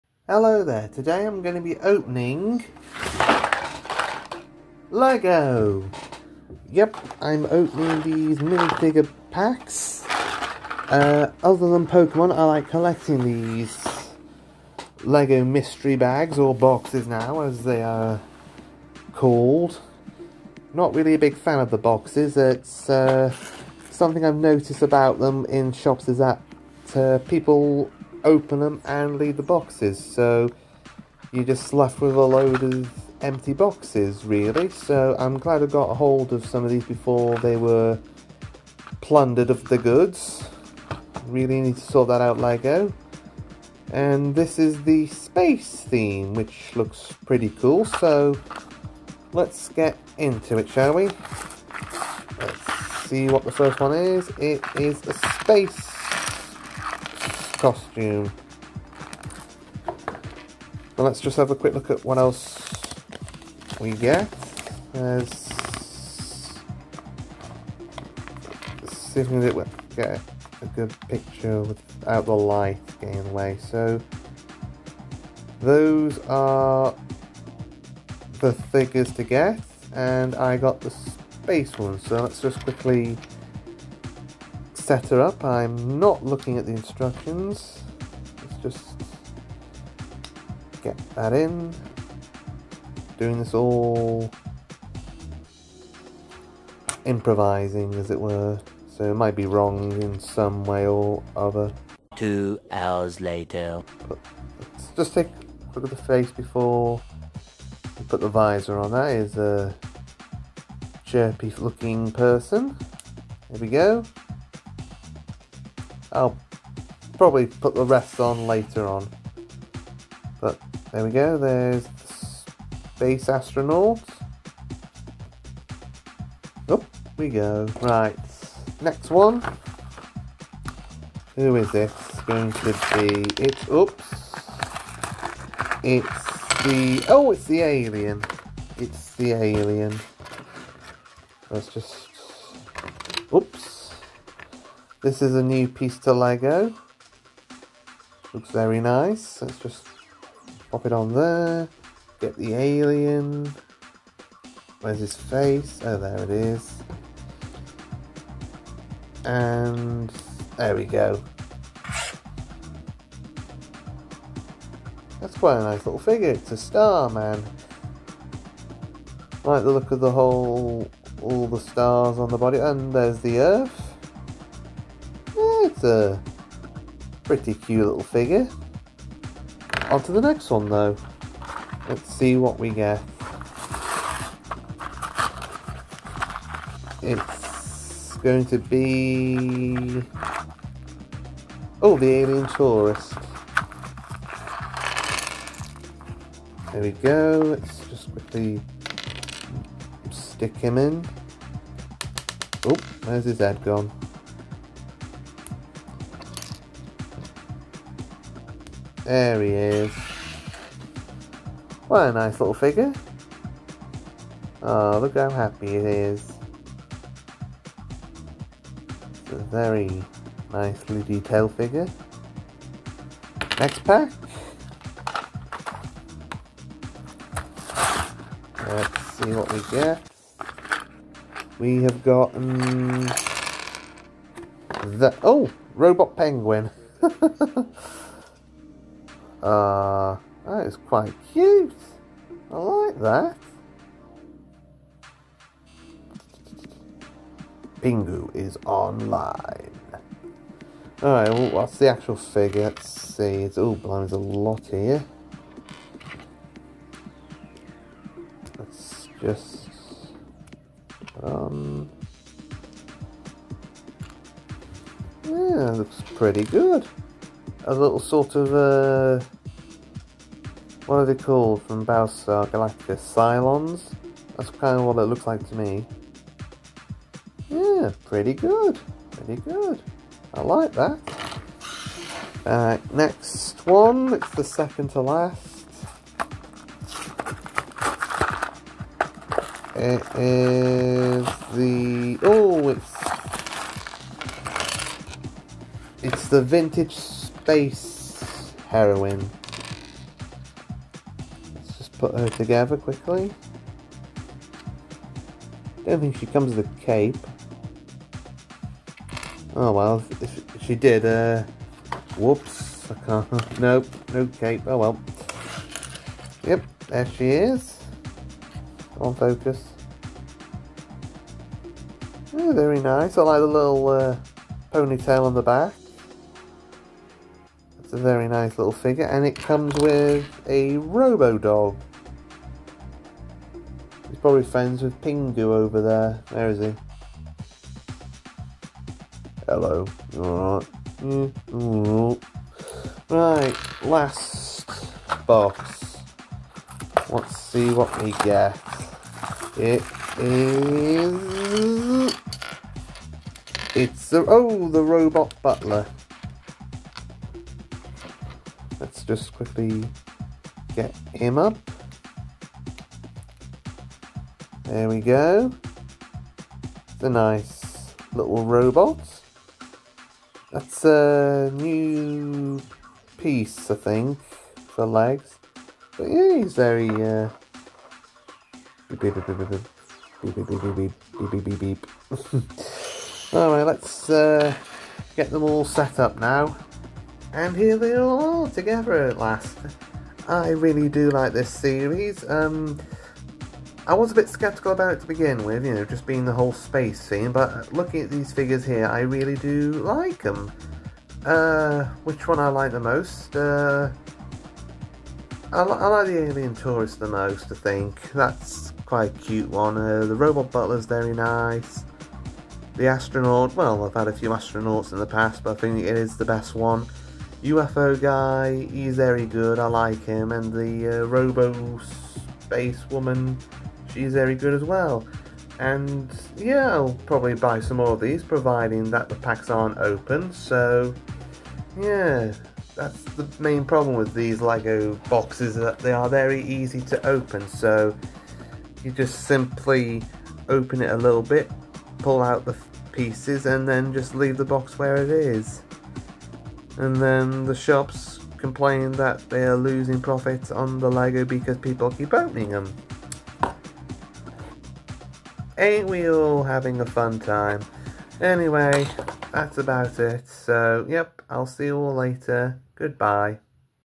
I Open Lego Space Minifigures
I open six Lego Space minifigures. Will I be lucky and get no repeats or will I get any that haven't been tampered with?